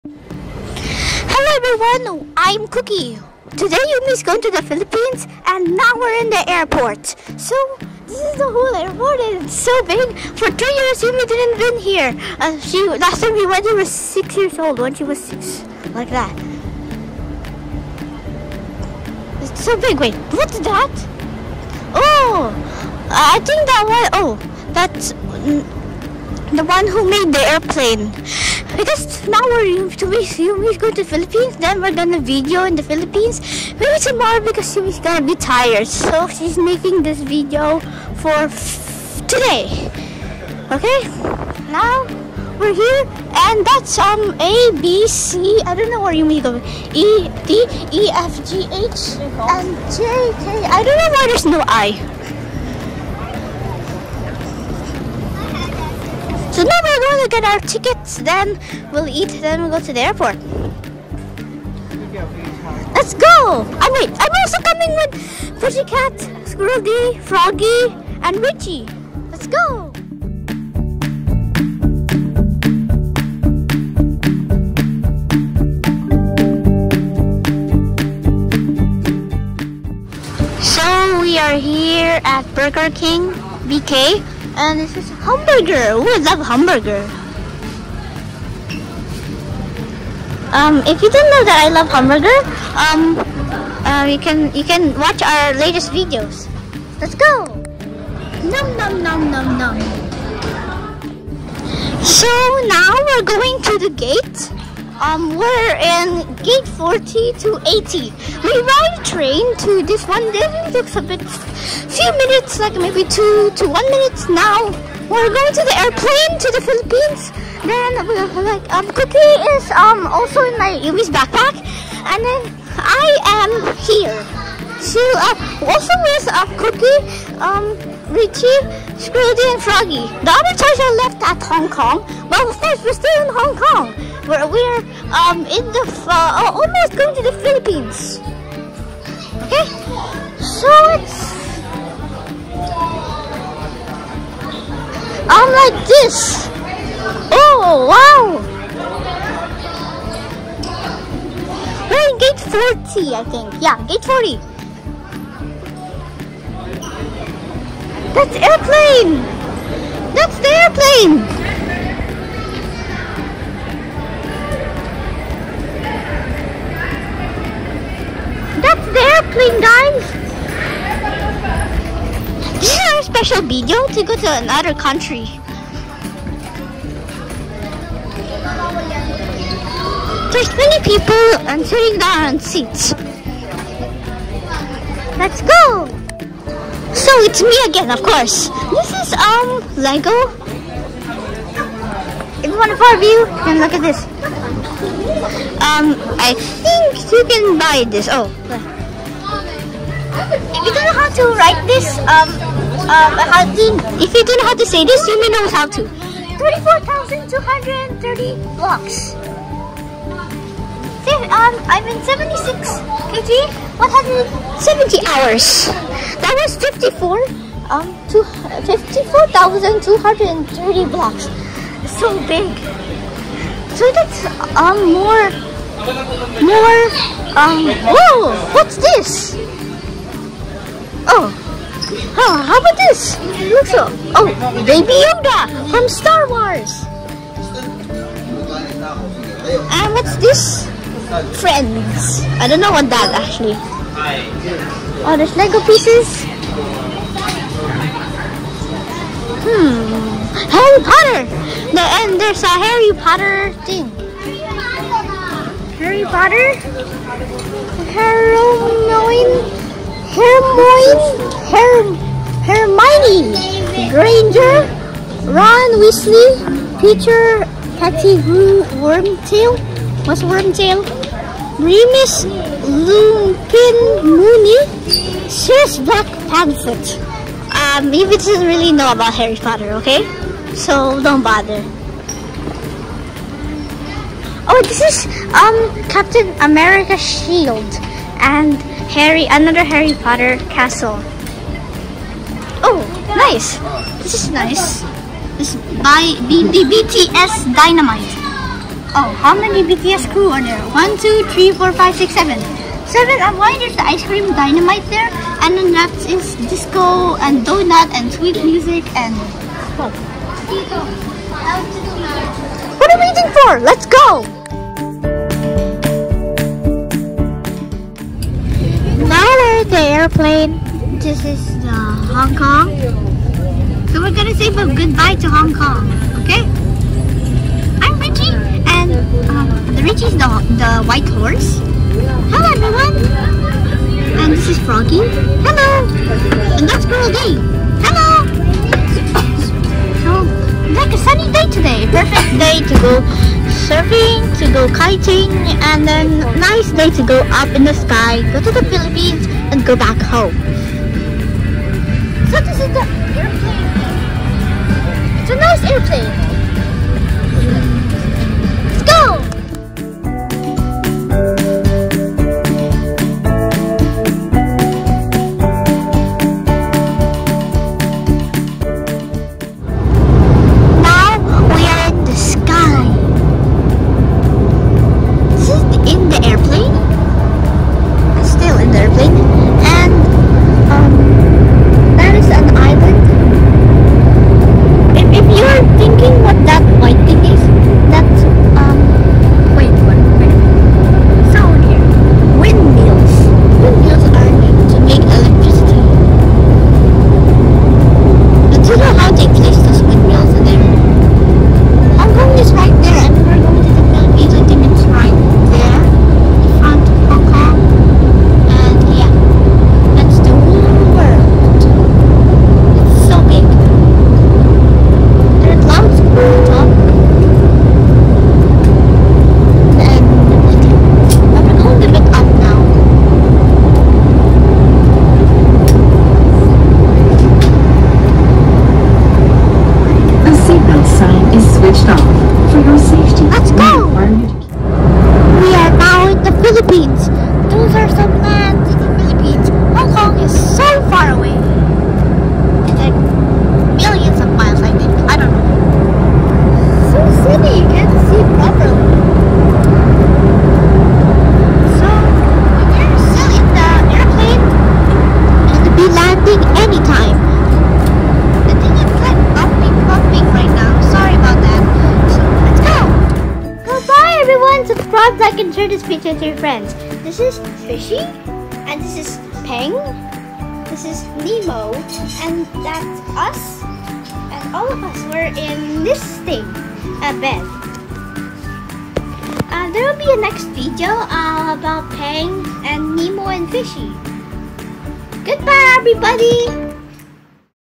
DAF SPECIAL | Trip to the Philippines 🇵🇭!(one show,Special)
Pushycat,Cooky,Richy,Squirrel.D and Froggy:

   Hi everyone!We are part of the DAF Crew , And we are going to the Philippines 🇵🇭!I hope you enjoy playing with our new friends PENG,NEMO AND FISHY!

   Fishy is going to stay in the Philippines 🇵🇭 .Because he is a toy in the Philippines 🇵🇭 ,But , Do need to worry, We will also come back to the Philippines 🇵🇭 On December!

Good bye!Jesus loves you!